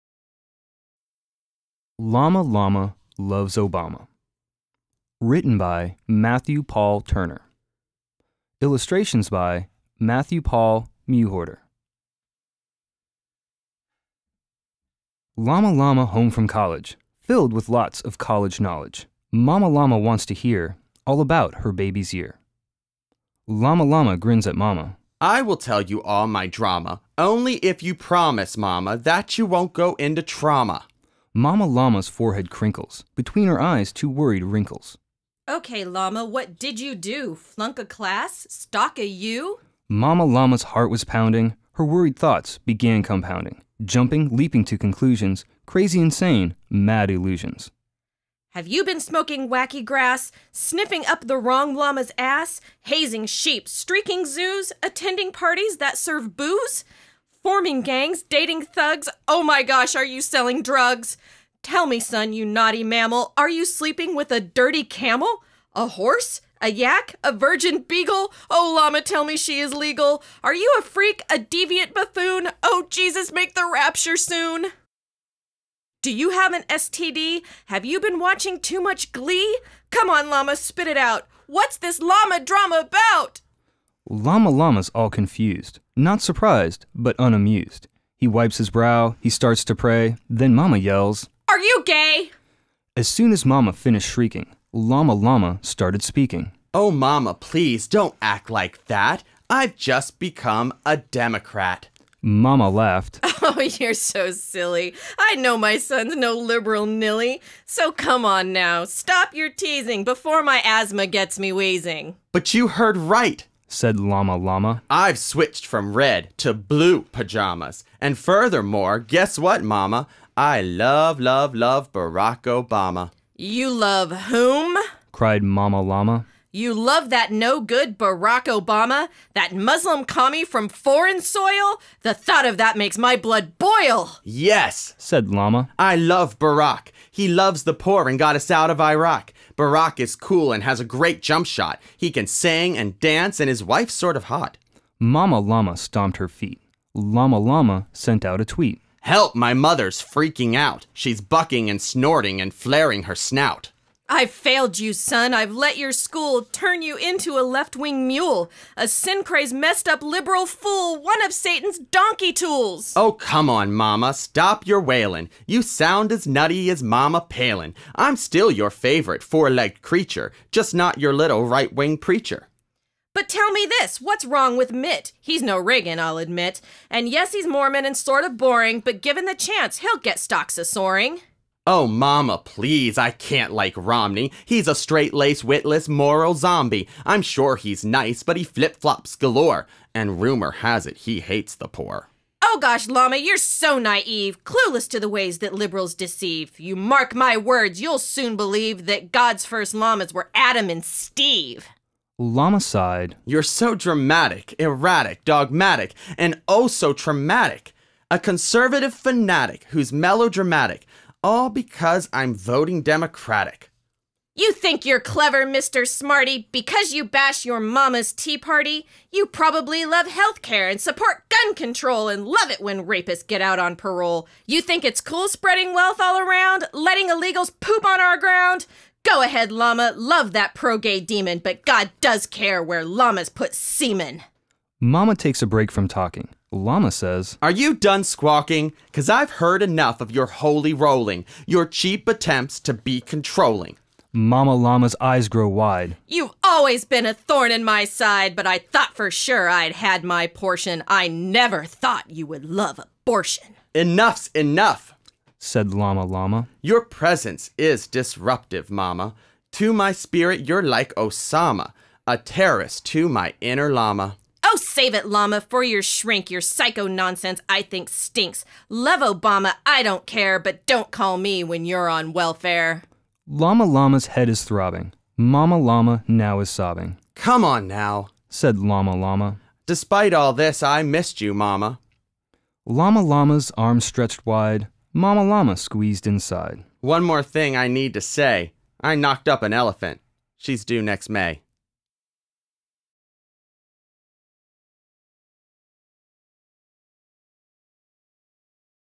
Llama Llama Loves Obama: A Children's Book For Adults
"Llama Llama Loves Obama" is a political satire based on the well-loved "Llama Llama" children's book series. It was written by Matthew Paul Turner and illustrations are by Matthew Paul Mewhorter.

The book is being performed by Sharideth Smith, Geoff Piller, and Matthew Paul Turner.

You can download the book at Amazon.com